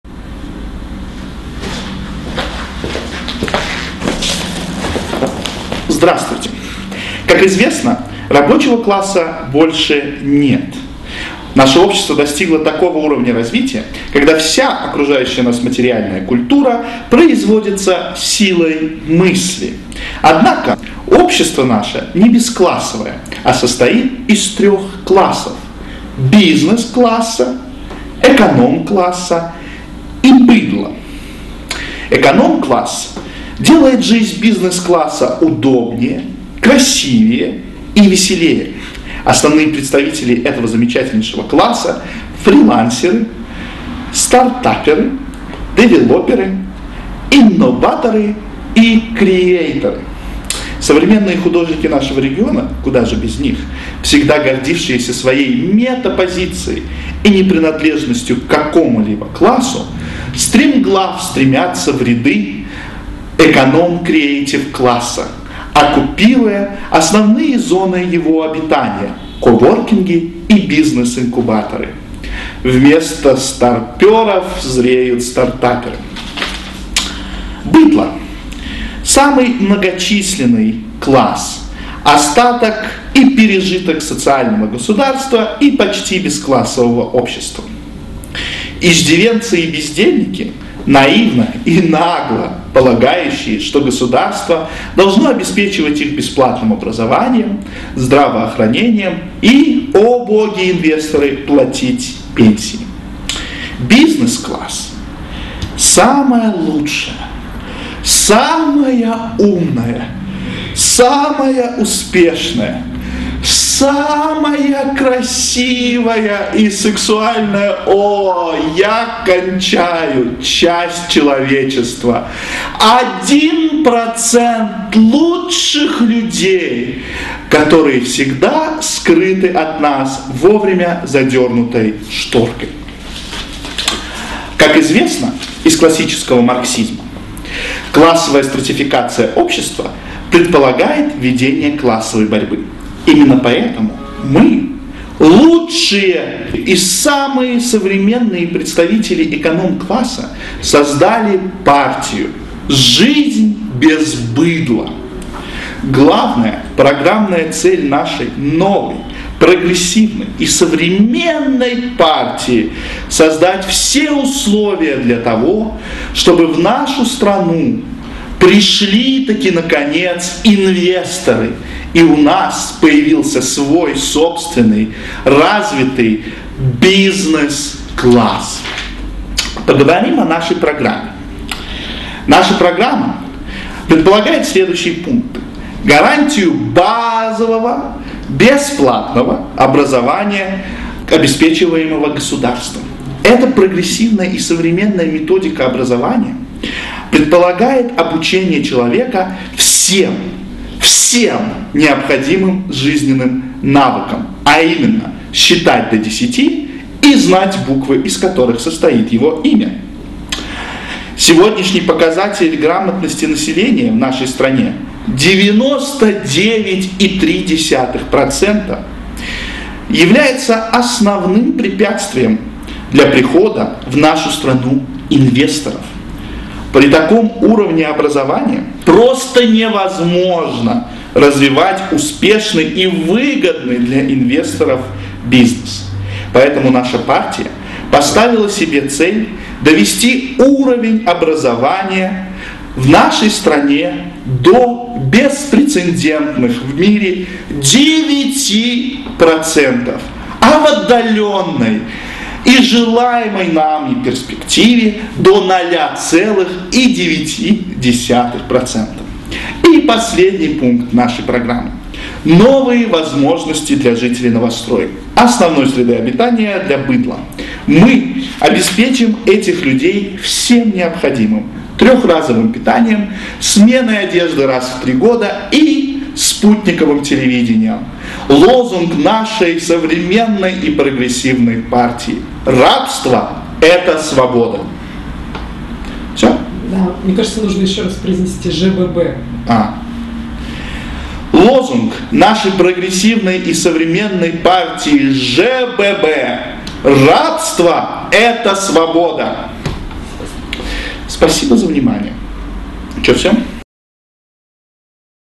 ХАМтуалы-2. Предвыборный ролик партии ЖББ
"Хамтуалы" - наши Окна РОСТА. Мы решили развить и довести до логического завершения заявления и программы некоторых партий, баллотирующихся в данный момент в городской совет Бишкека. Так появилась наша партия - самая современная партия ЖББ! Итого, смотрите предвыборный ролик прогрессивной, инновативной, современной партии "Жизнь Без быдла"! Кроме того, вы познакомитесь с современной классовой теорией — прогрессивной и инновативной, — в отличие от устаревшей и формальной теории Маркса.